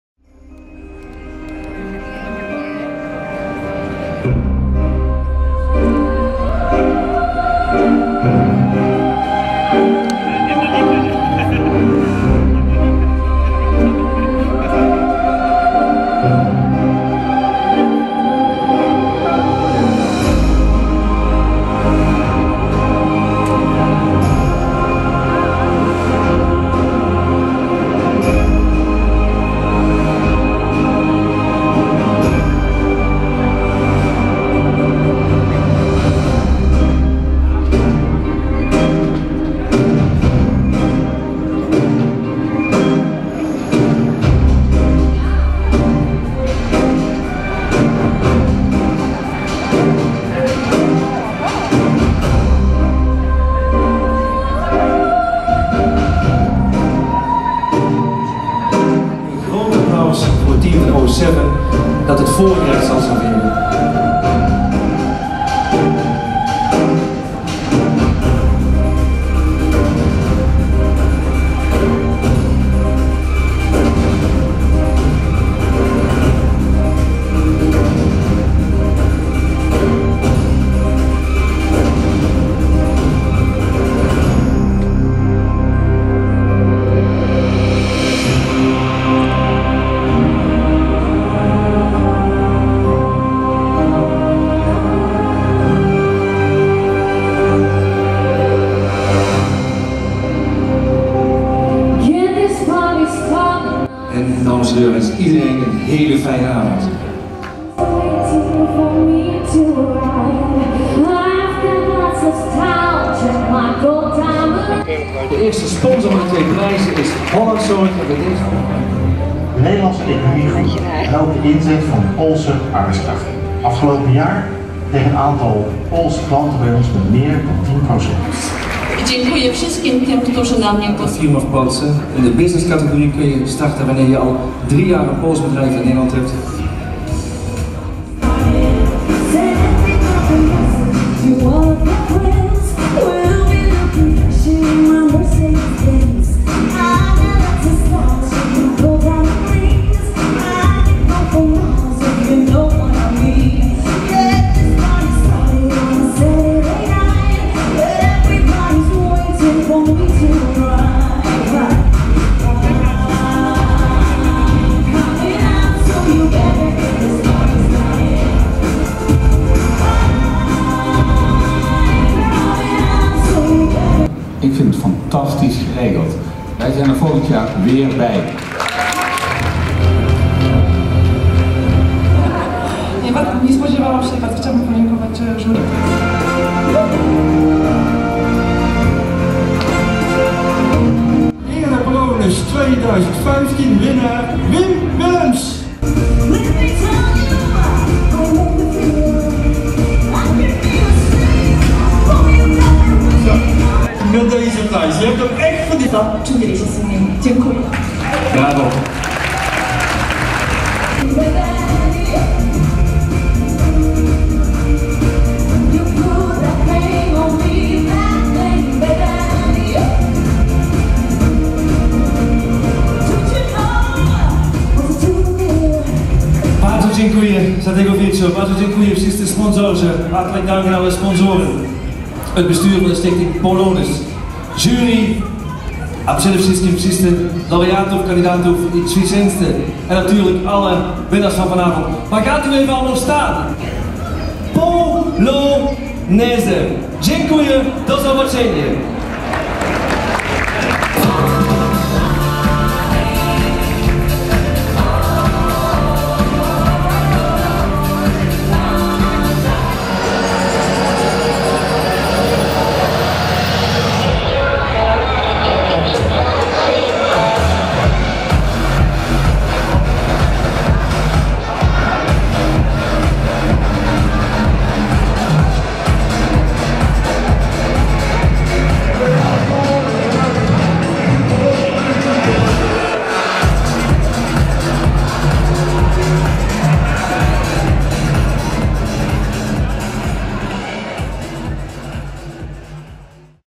Polonus2015
Een korte impressie van het feest van Polonus op 21 november. Wil je ook een impressie van je evenement of van je bedrijf, kijk dan op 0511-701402